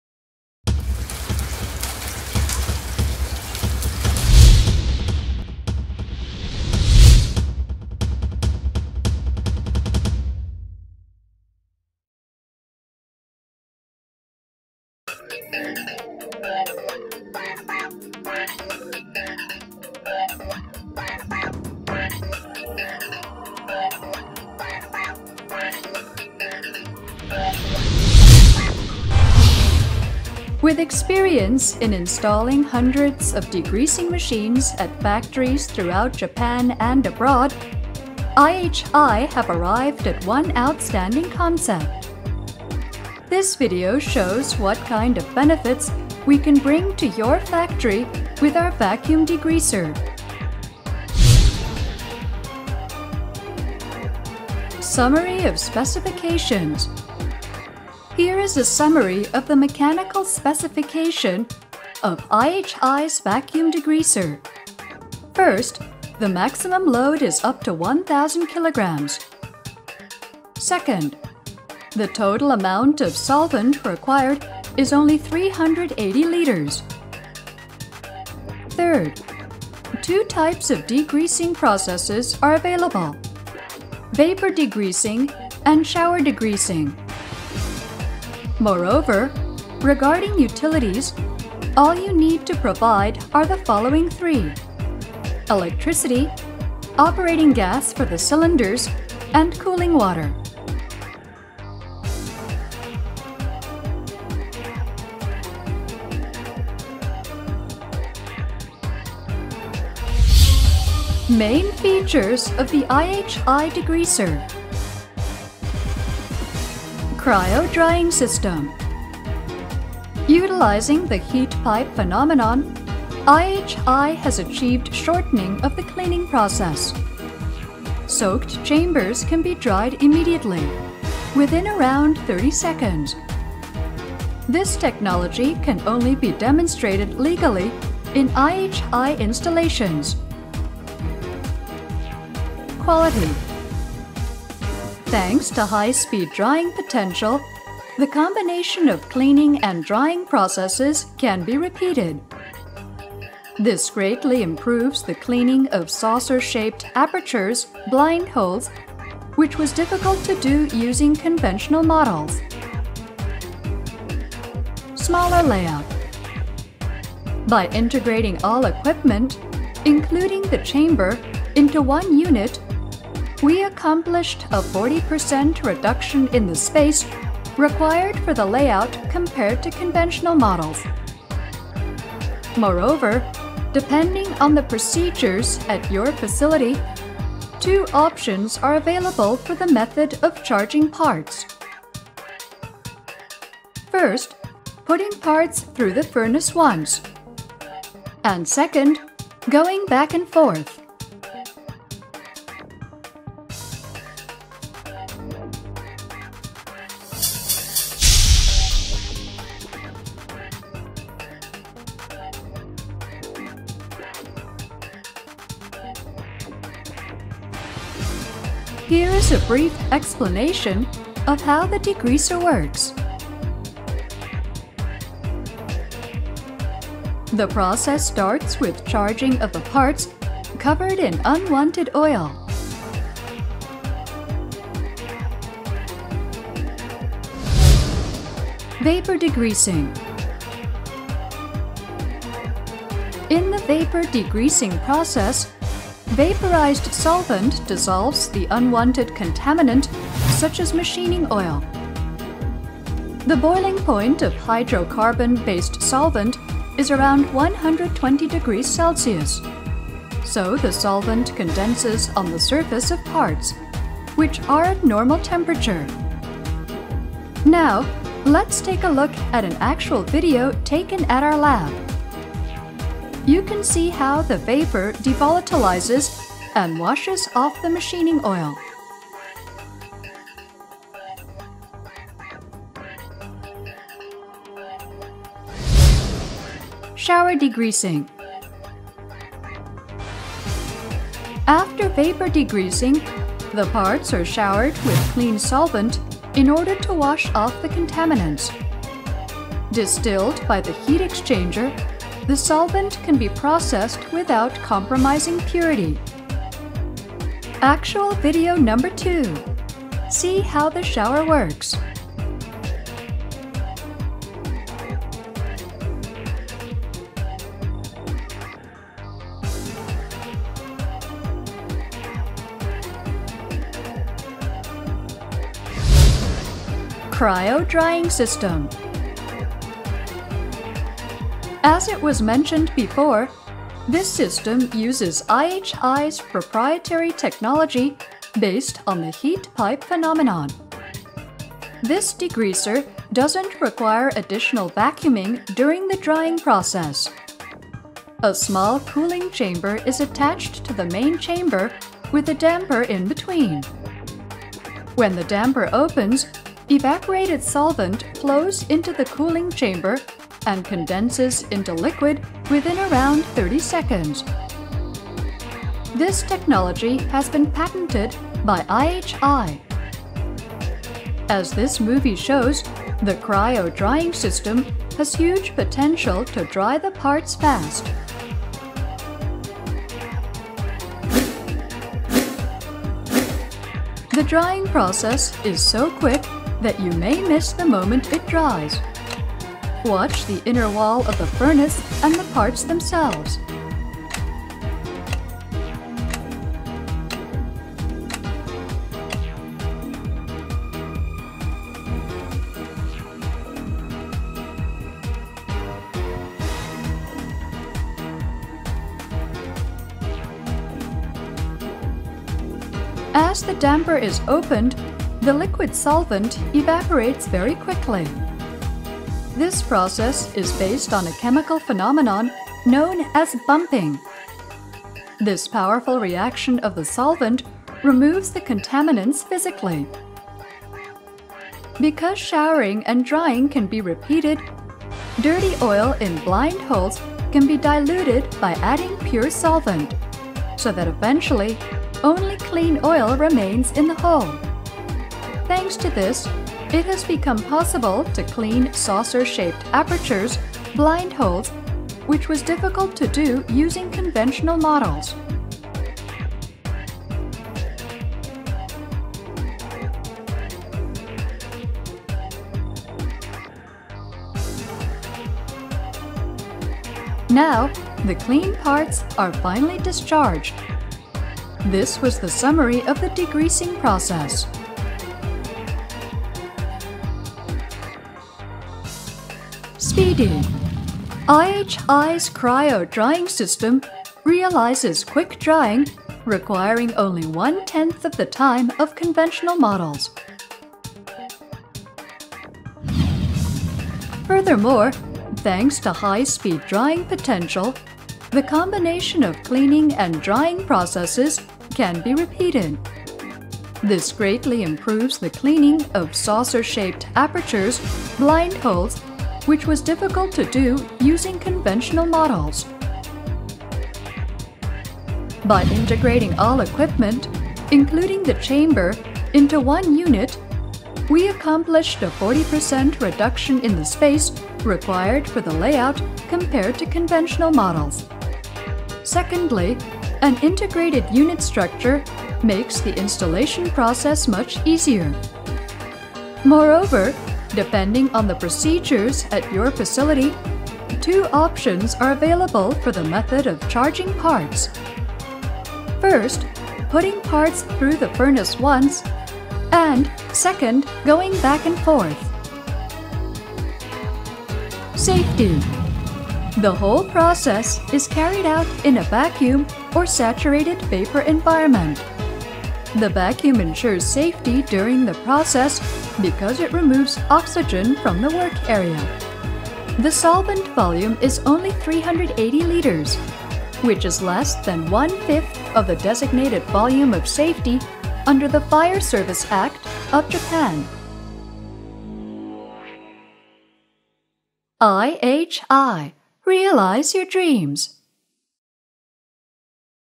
IHI Ecological vacuum degreaser IWV-34C
IHI offers new type of vacuum degreaser with low energy consumption.Our "Cryo Drying System"reduces drying time in the cleaning process.